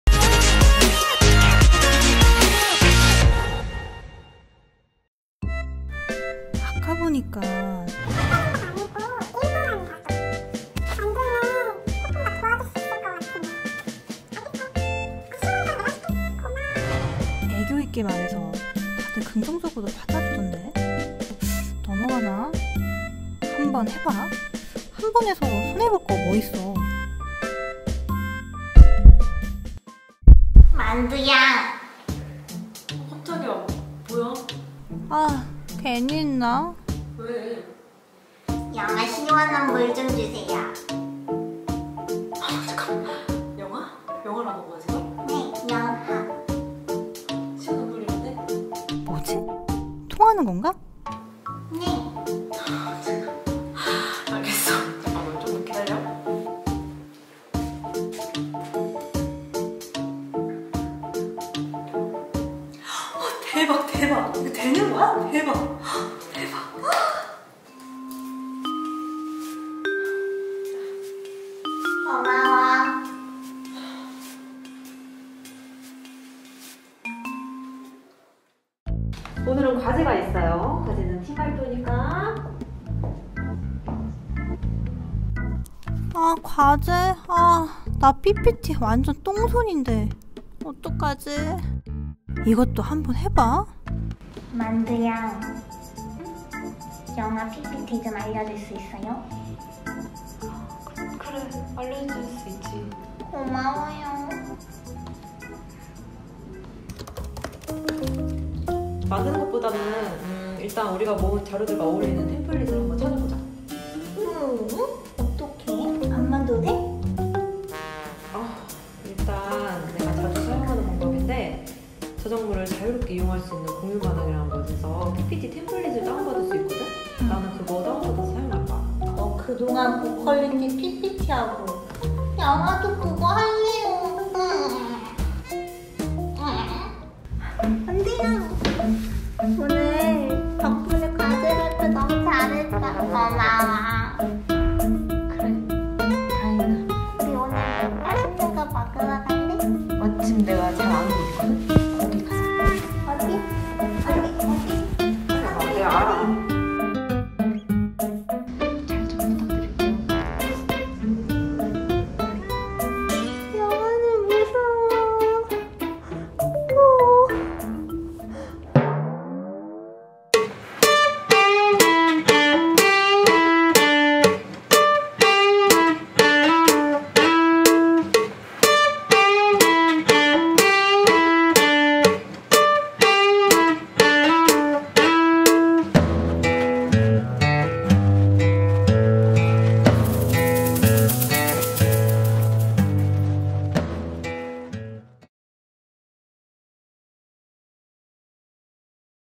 [저작권 공감영상 제작단] 오늘의 저작권 l 특급 애교로 과제 해내는법 (feat.나문희의 애교)
특급 애교로 과제를 해낸다고!?

영하가 만두에게 부탁하는 것은 무엇일까요?(●ˇ∀ˇ●)
과연 애교가 만두에게 통할 것인가!

지금 바로 영상으로 확인해 보세요~